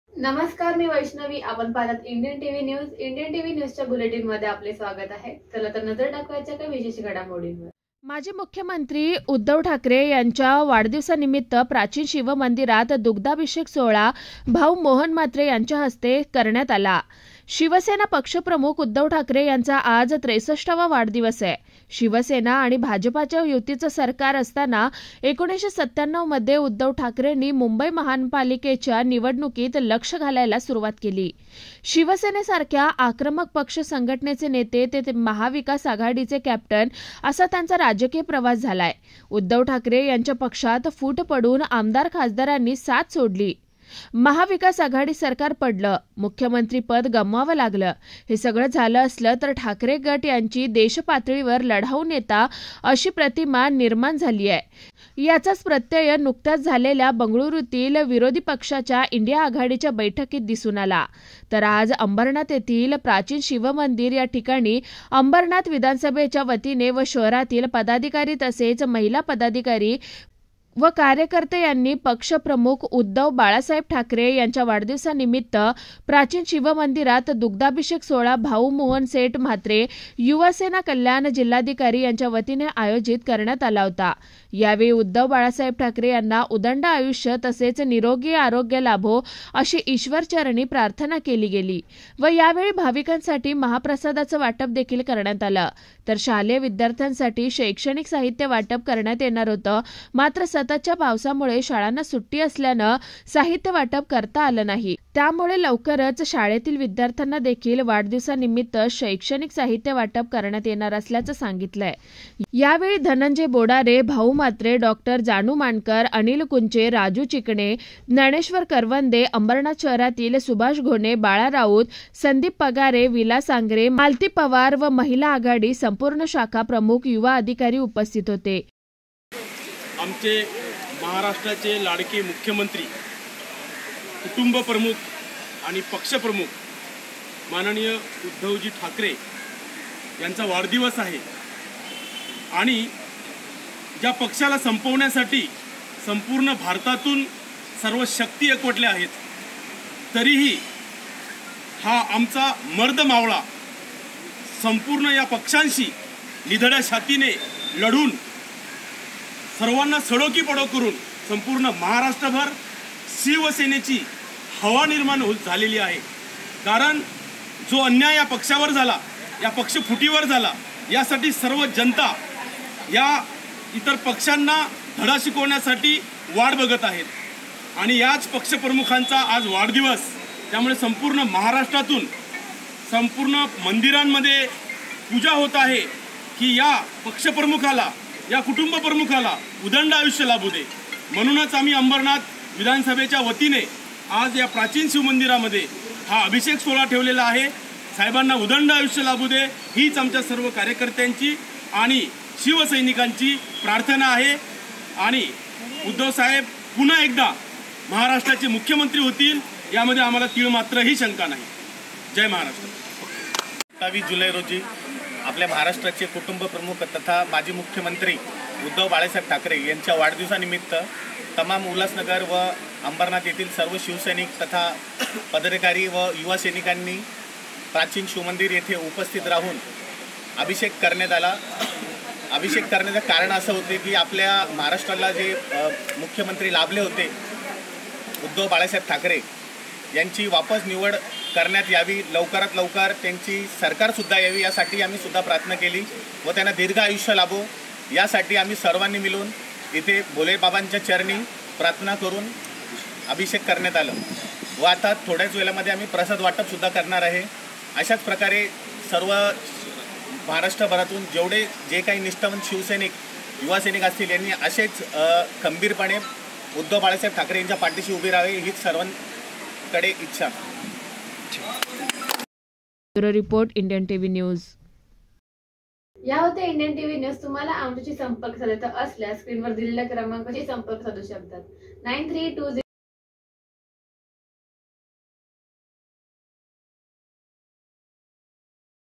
मा.मुख्यमंत्री उद्धव ठाकरे यांच्या वाढदिवसानिमित्त प्राचीन शिव मंदिरात येथे दुग्धाभिषेक सोहळा संपन्न
मा. मुख्यमंत्री उद्धव ठाकरे यांच्या वाढदिवसानिमित्त प्राचीन शिव मंदिरात भाऊ मोहन म्हात्रे  यांच्या हस्ते दुग्धाभिषेक सोहळा संपन्न...